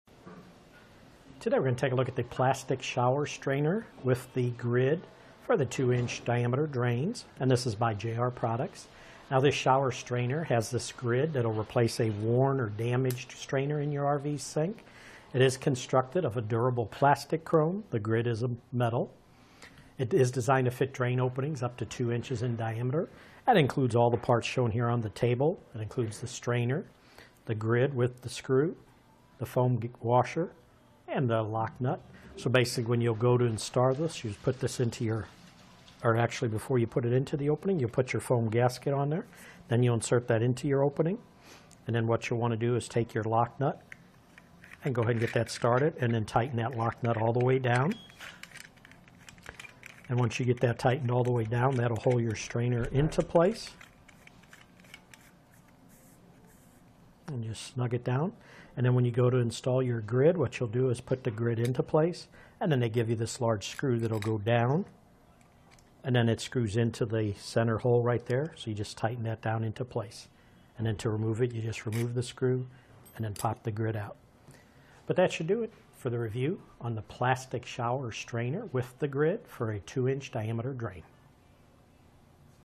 etrailer | Comprehensive Review: RV Shower Drain with Strainer
Click for more info and reviews of this JR Products RV Showers and Tubs:

Today we're going to take a look at the plastic shower strainer with the grid for the two inch diameter drains. This is by jr products. This shower strainer has this grid that'll replace a worn or damaged strainer in your RV sink. It is constructed of a durable plastic chrome. The grid is a metal. It is designed to fit drain openings up to two inches in diameter, and includes all the parts shown here on the table, and includes the strainer, the grid with the screw, the foam washer, and the locknut.When you'll go to install this, before you put it into the opening, you'll put your foam gasket on there.

Then you'll insert that into your opening. Then what you'll want to do is, take your lock nut and go ahead and get that started. Then tighten that lock nut all the way down. Once you get that tightened all the way down, that'll hold your strainer into place. Snug it down.

then when you go to install your grid, what you'll do is put the grid into place. then they give you this large screw that'll go down. Then it screws into the center hole right there. You tighten that down into place. Then to remove it, you remove the screw and then pop the grid out.

But That should do it for the review on the plastic shower strainer with the grid for a two inch diameter drain.